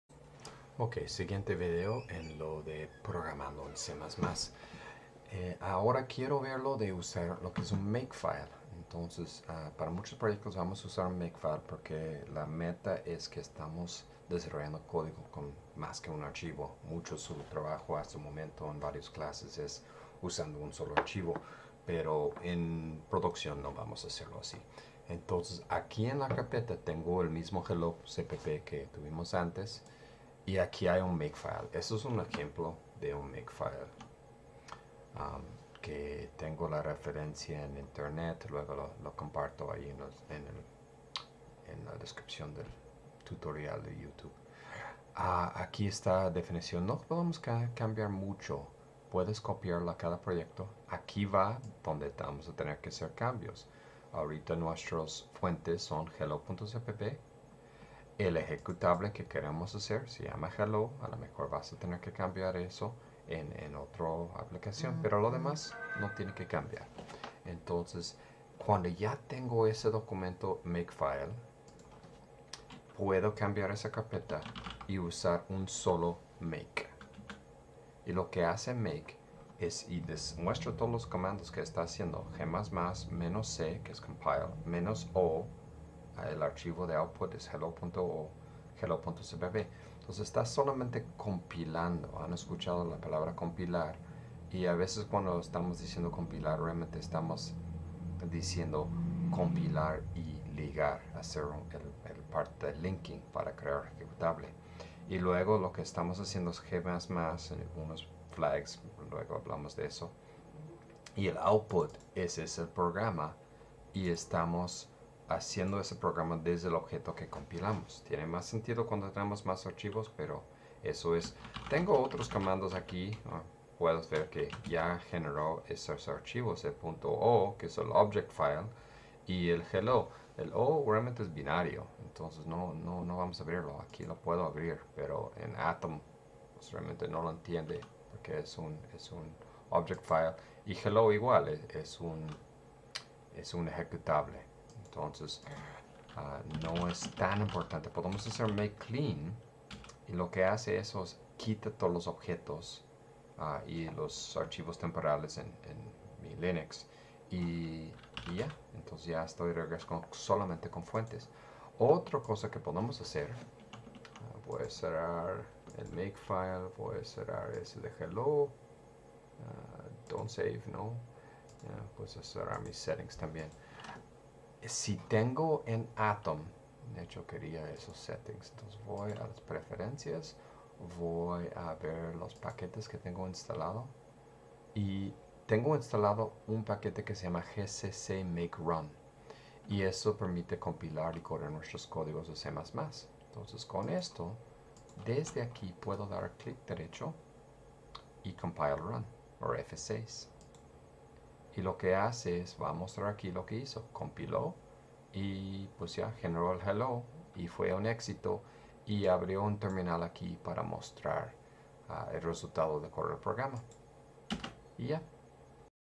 Programando en C++ - Makefile
Como usar un Makefile básico.

Programando en C++ es una serie de videos cortos principalmente para mis alumnos del Tecnológico de Monterrey pero un recurso abierto para todos.